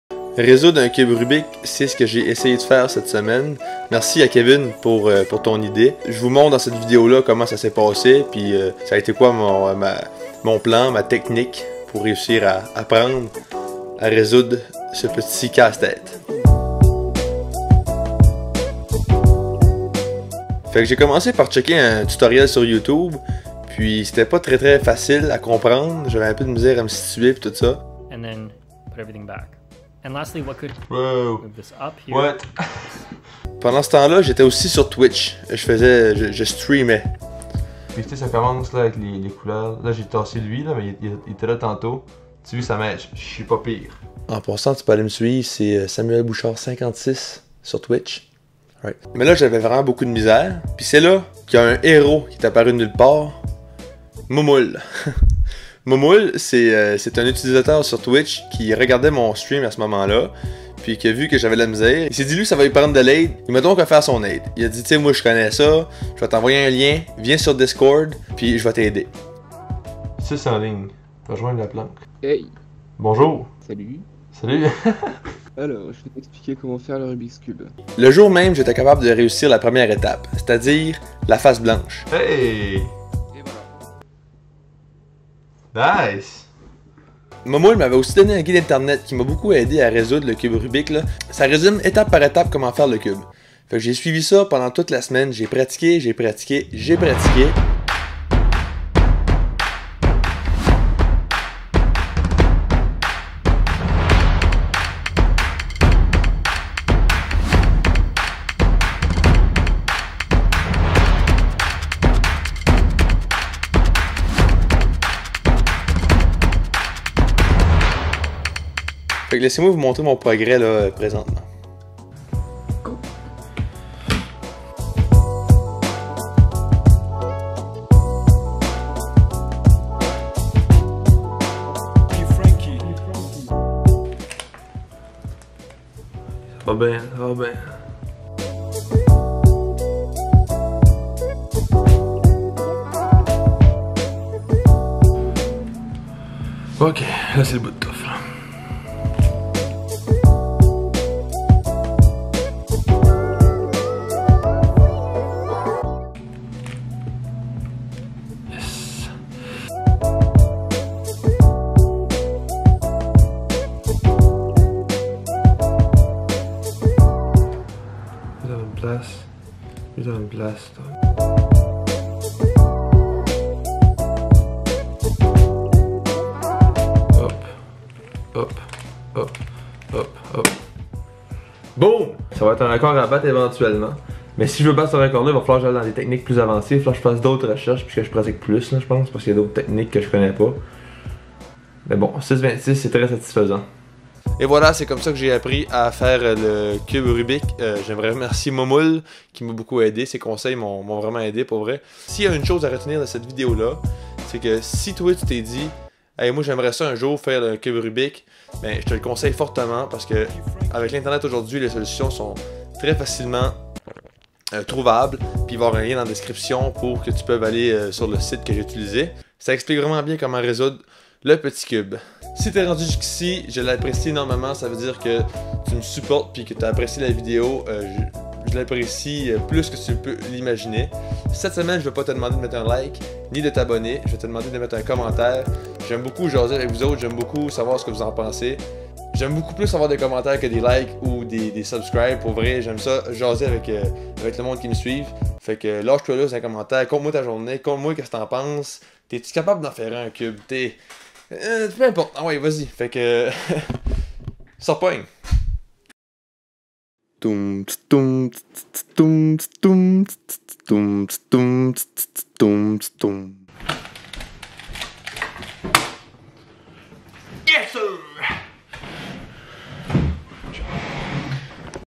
Comment faire le RUBIK'S CUBE? C'est ce que j'ai appris
Comment faire le Rubiks cube? Je vous montre comment je l'ai appris. Avec l'aide de Youtube, Moumoule et un site internet tout simplement.

Apprendre à faire le rubiks cube n'est pas si difficile, j'ai probablement mis environ 10h de pratique avant d'être capable de le faire tout seul, sans guide sur internet. Il sufffit de suivre différente combinaisons de mouvements au bon moment.

Le cube rubik est un casse tête très populaire, tout le monde à déjâ envie d'apprendre à le faire, je te le recommande, c'est très satisfesant et plaisant d'apprendre à résoudre ce casse-tête.

Me suivre:
Tik Tok et Twitch: samuelbouchard56

Laissez-moi savoir dans les commentaire si vous savez comment résoudre le cube rubik.